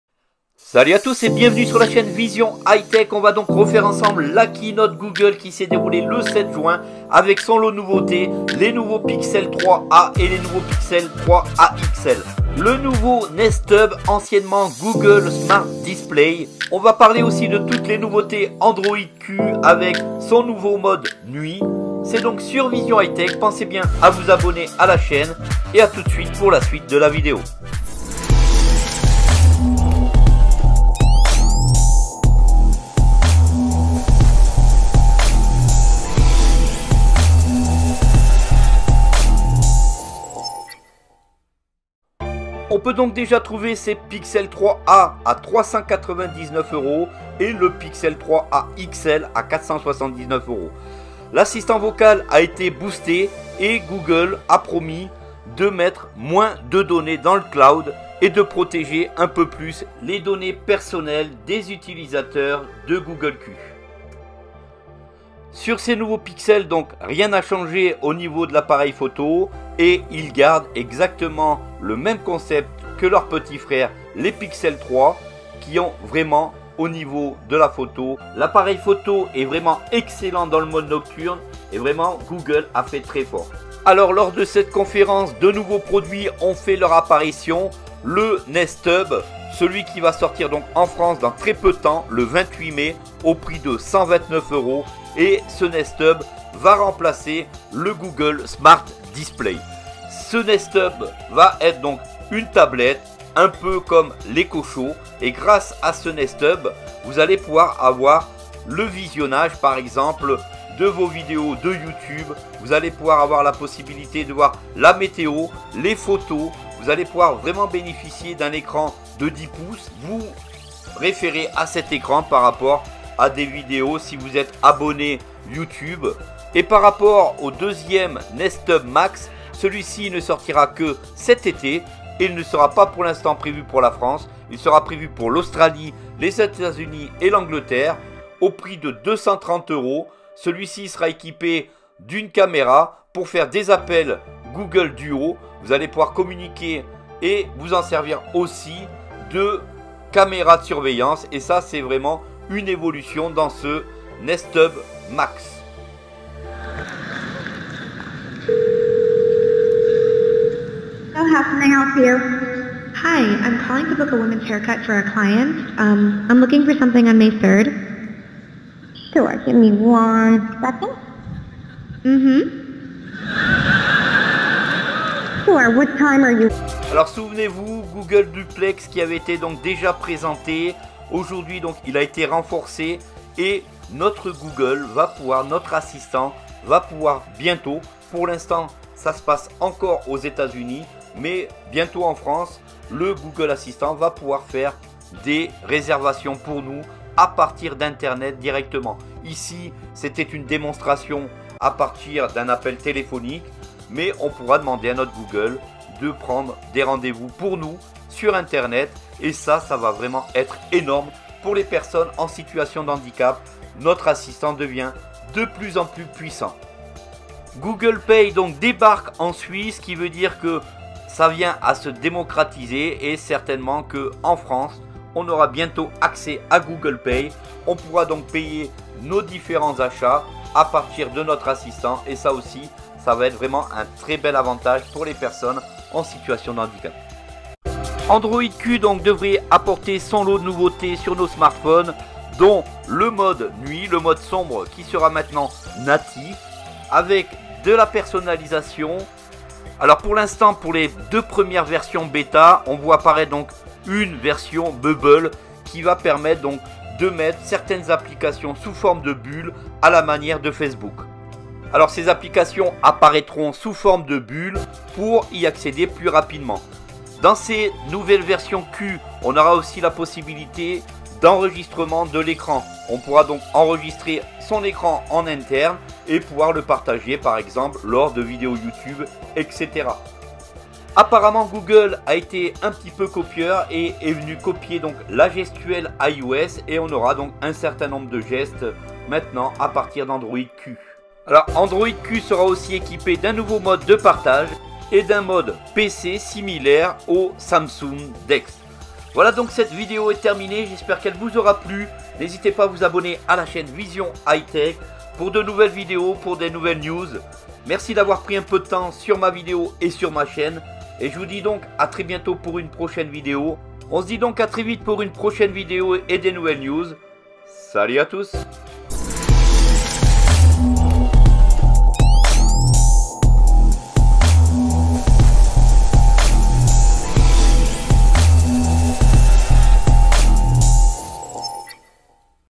Nouveauté Android Q Keynote Google I/O Pixels 3 A Nest HUB Assistant Google Toues Les News En Vidéo!
Salut Utilisateur d'Android voici un petit bilan sur la Keynote Google I/O 2019 avec toutes ses nouveautés Pixel 3 A et Pixels 3 A XL, le tout nouveau Nest Hub, les nouveautés Android Q et quoi de neuf avec notre Assistant Google et Duplex voilà merci de vous abonner ça fait toujours plaisir @+.